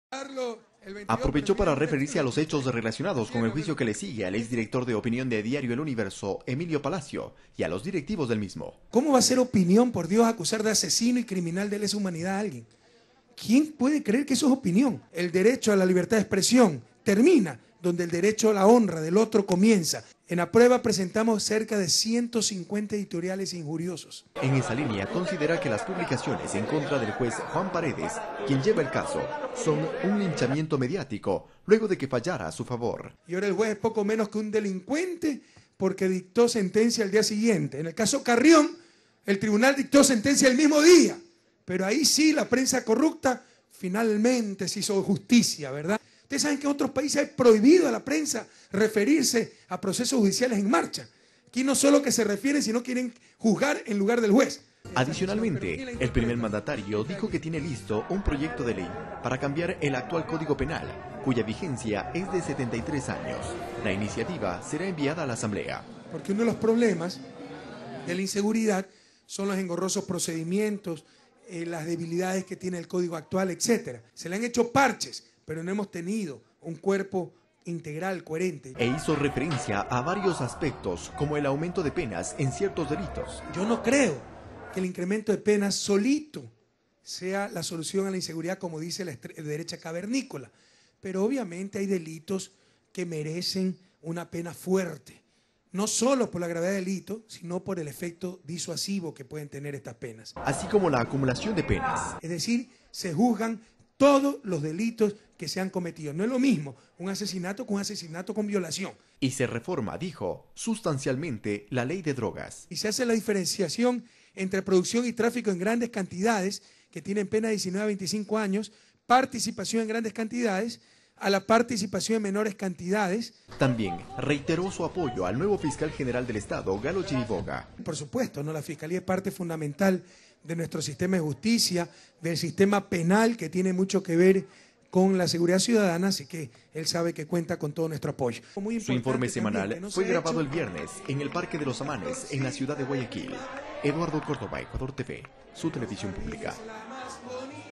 ECTV NOTICIAS-RESUMEN CADENA SABATINA 23 DE JULIO
Durante el enlace sabatino, el Presidente Correa, dedicó un importante espacio del mismo para explicar la situación jurídica con el diario El Universo y Emilio Palacio. Además, anunció que enviará a la Asamblea un proyecto de ley para reformar el código penal.